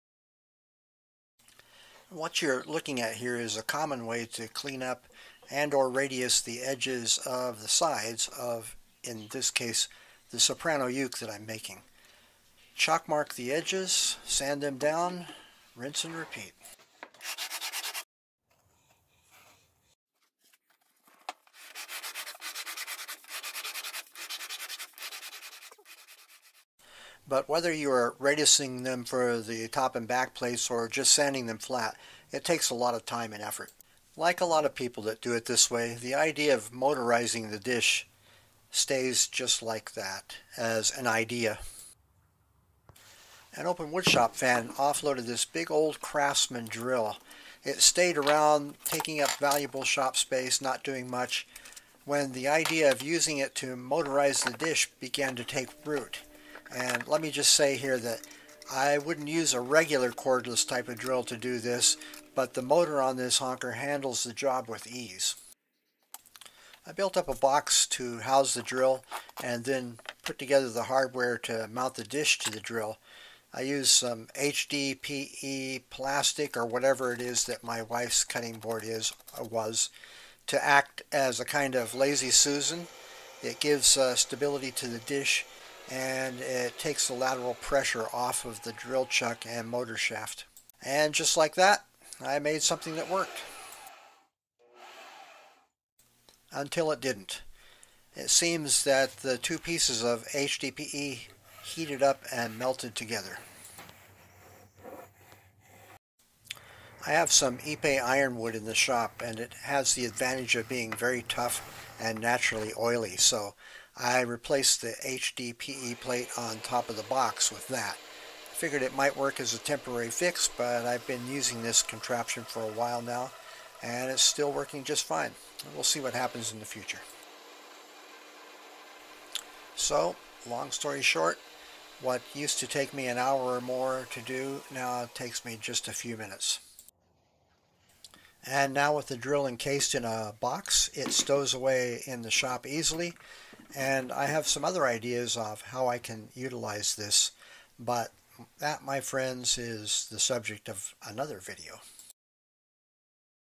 Electrifying a Dish Sander
A dish sander is typically used by luthiers to radius sand the edges of the sides of guitars, ukuleles and other stringed instruments. Often times it is done manually but I made one simply that is motorized, saving time and labor.

And BTW, I know those corners look pretty wicked when the dish is spinning, but that is mostly a camera trick and the dish is not spinning as fast as it appears to be spinning. Nevertheless, a round dish is being made because the present one is part of my go-bar deck, and making another one for the sander means that I won't have to take apart the go-bar deck everytime I need to radius sand.